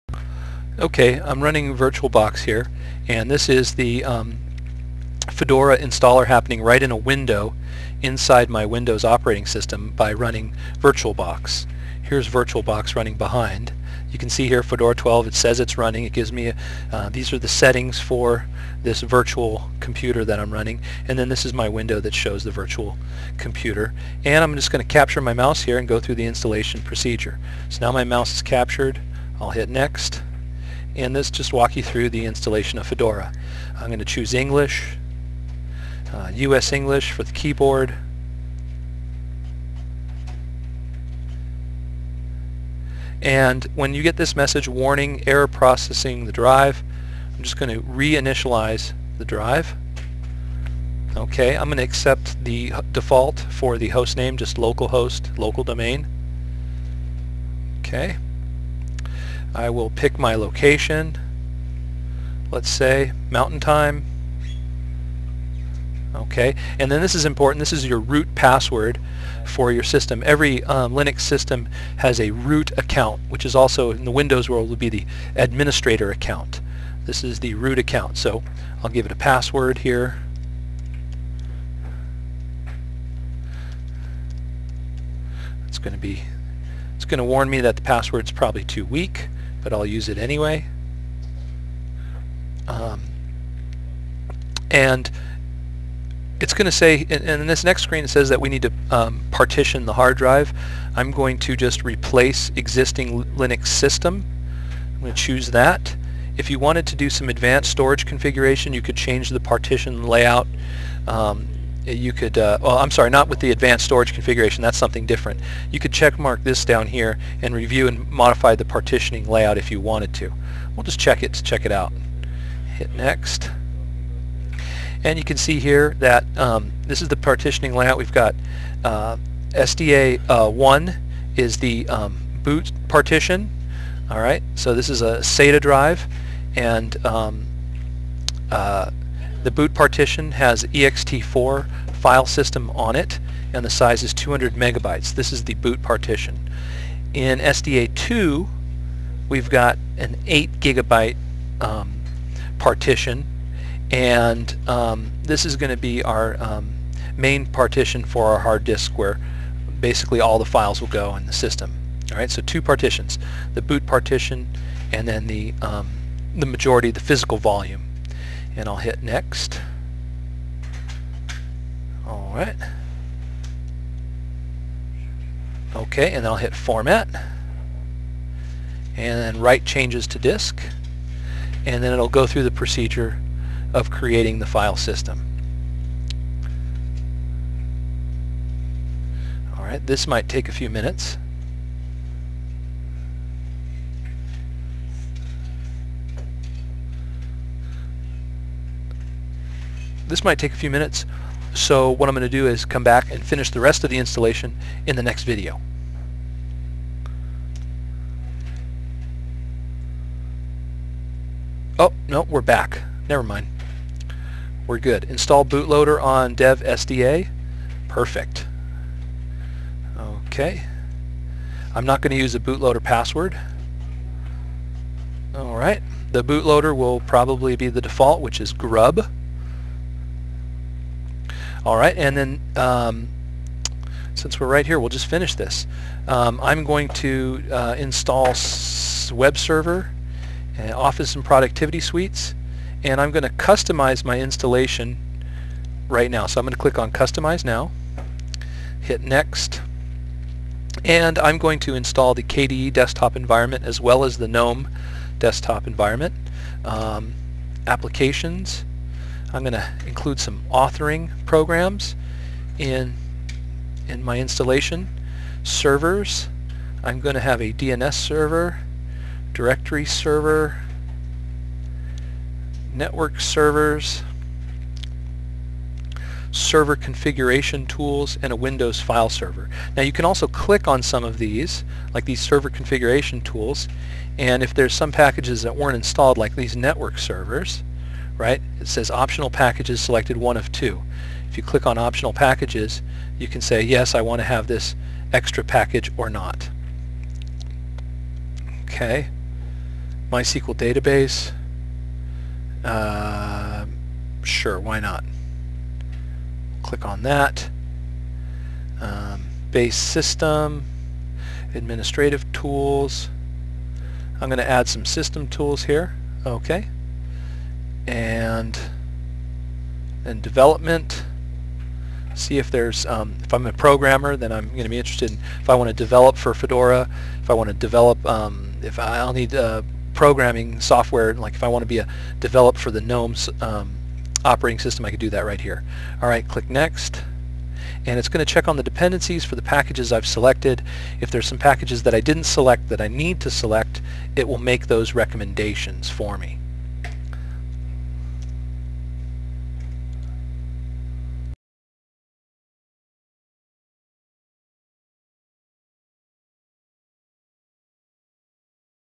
Installing Fedora with VirtualBox -part2
A tutorial on installing Fedora using VirtualBox